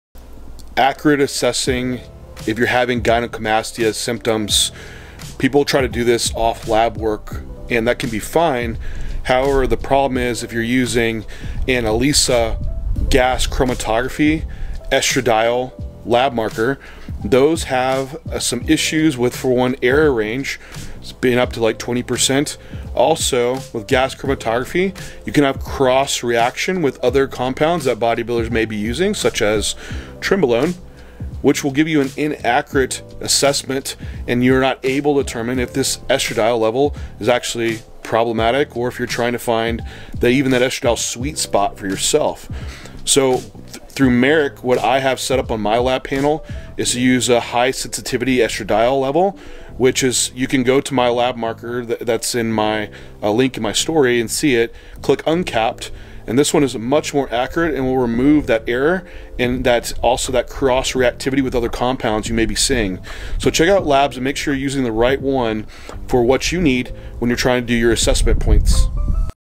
Inaccuracy of Estradiol for gyno assessment Most standard labs use gas chromatography mass spectro
Inaccuracy of Estradiol for gyno assessment

Most standard labs use gas chromatography mass spectrometry for estradiol which has a larger error range and many other PEDs taken may cross react.

Example is I see many cases of pre contest labs and E2 is high, but it is actually trenbolone cross reacting in the test to show up as estradiol.

For accuracy of assessment an high performance liquid chromatography (HPLC) would be preferred like what is given with a high sensitivity estradiol serum test.

Check out the link in my story for my suggest labs for bodybuilding through Marek Health and click “uncapped” for the high sensitivity E2 test.